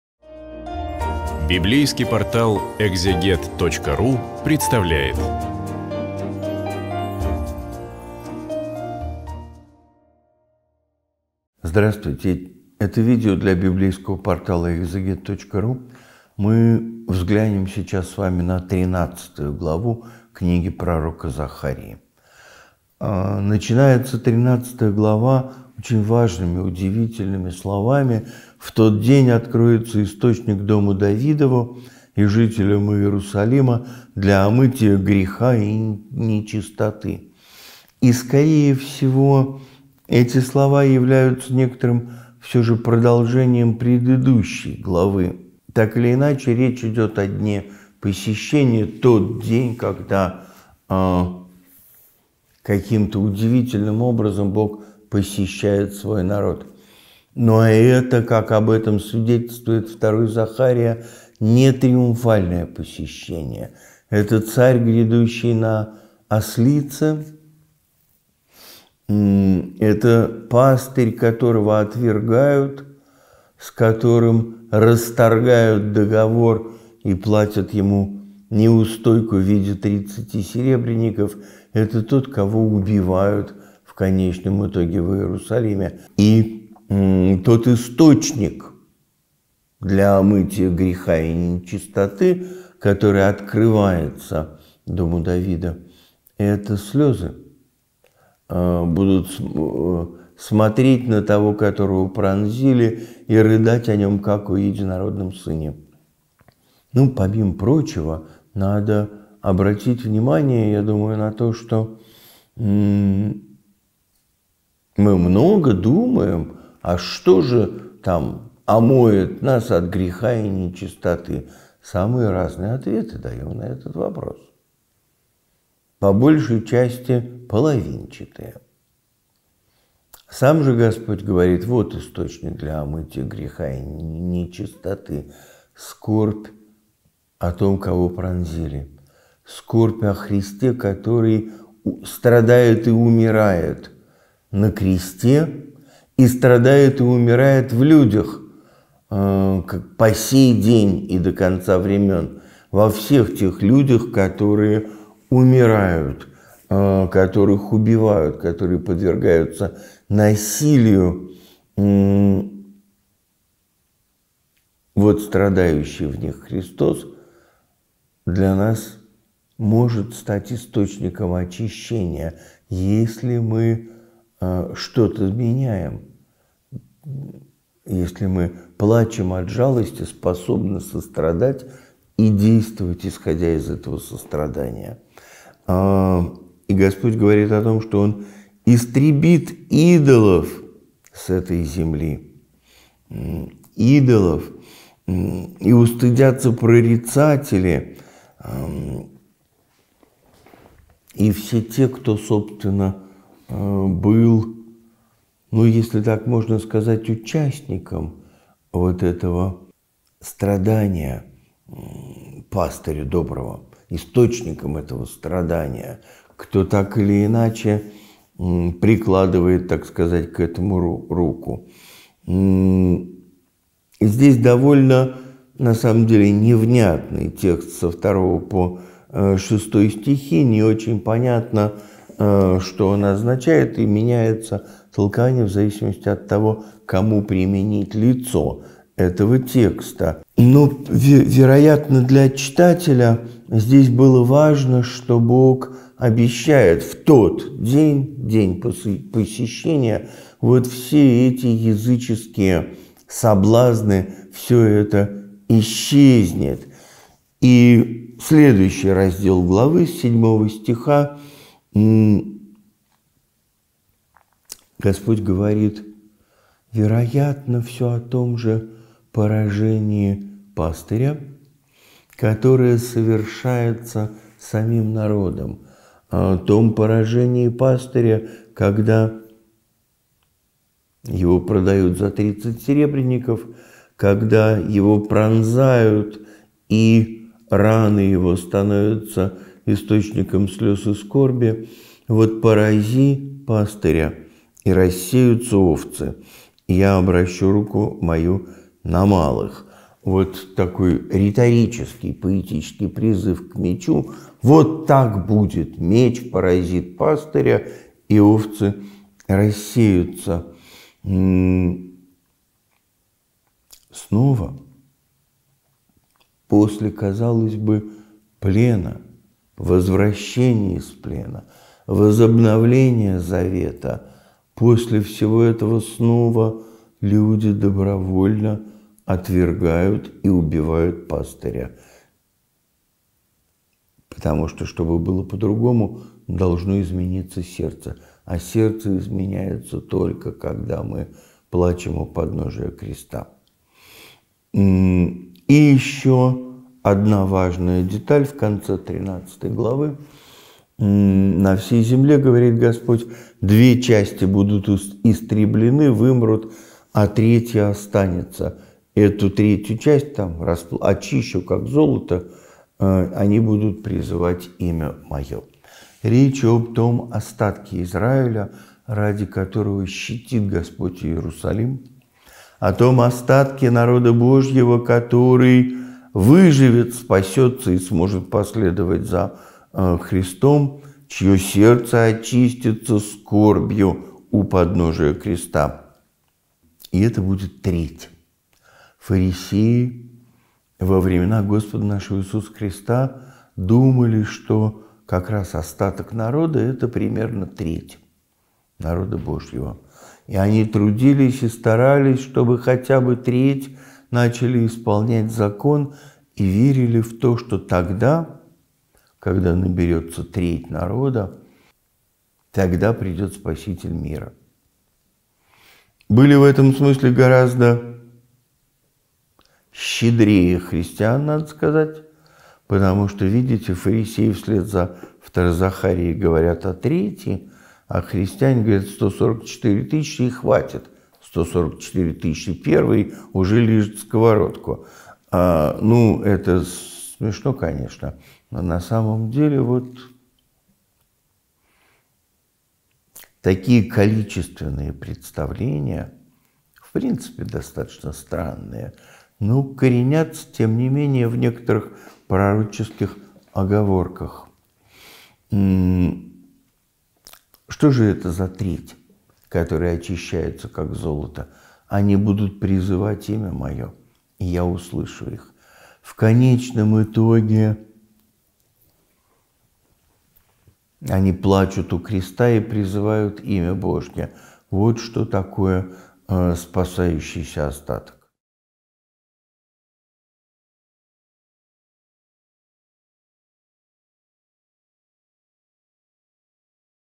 Книга пророка Захарии. Глава 13 "Порази пастыря и рассеются овцы!" Священник Антоний Лакирев
Команда Экзегет.

Библейский портал Экзегет вместе со священником Антонием Лакиревым продолжает изучение книги пророка Захарии.
Книга состоит из двух частей, написанных разными людьми в разное время и содержит апокалиптические пророчества. Пророк Захария принадлежал к знатному роду. Он написал первую половину книги в 520 г. до Р.Х.
В сегодняшнем видео обратимся к 13-й главе.

🔸  Источник дома Давидова – образ очищения
🔸  День Посещения
🔸  Низвержение идолов и истребление лжепророков
🔸  "Порази пастыря и рассеются овцы!"
🔸  Истребление тех, кто был виновен в смерти Пастыря
🔸  Остаток народа Божьего